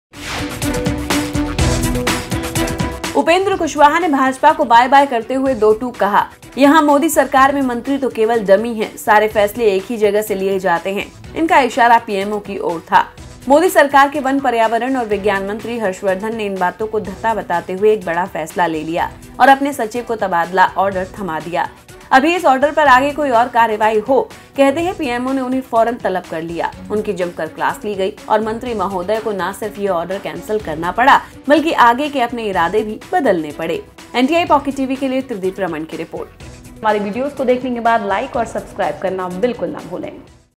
Upendra Kushwaha जो कह रह थे उसे PMO ने Harsh Vardhan को लताड़कर सच साबित कर दिया है | हर्षवर्द्धन
उपेंद्र कुशवाहा ने भाजपा को बाय-बाय करते हुए दो टूक कहा- ‘यहां मोदी सरकार में मंत्री तो केवल डमी है, सारे फैसले एक ही जगह से लिए जाते हैं, इनका इशारा पीएमओ की ओर था, मोदी सरकार के वन-पर्यावरण और विज्ञान मंत्री हर्षवर्द्धन ने इन बातों को धत्ता बताते हुए एक बड़ा फैसला ले लिया और अपने सचिव को तबादला ऑर्डर थमा दिया...